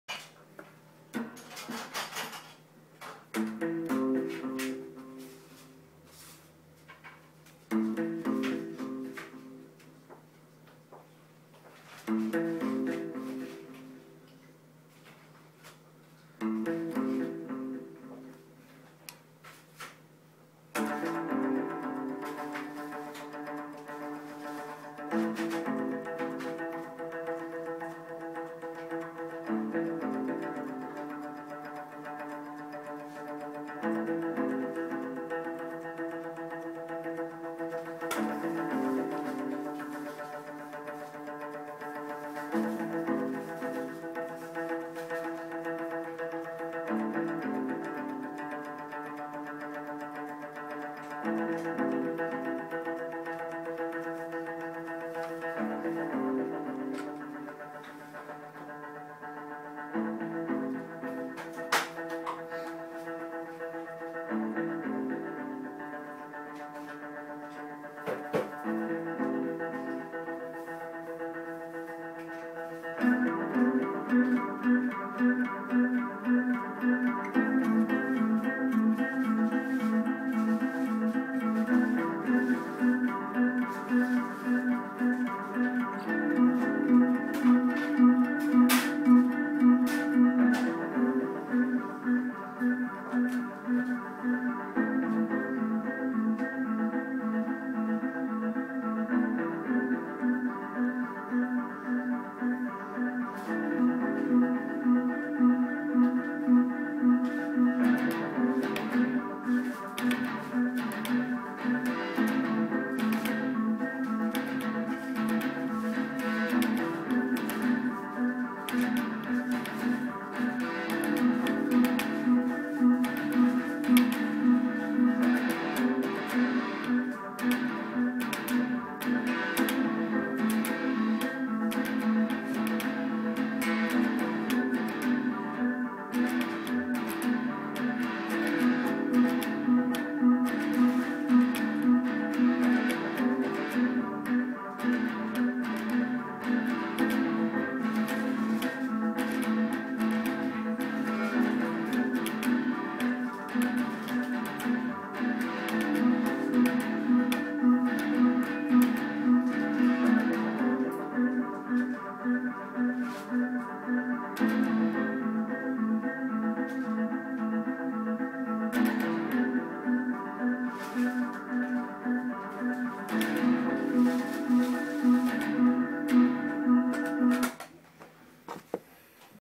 Hypnotic Guitar Loop
I was feeling this hypnotic loop when I was jamming today so I decide to post it up.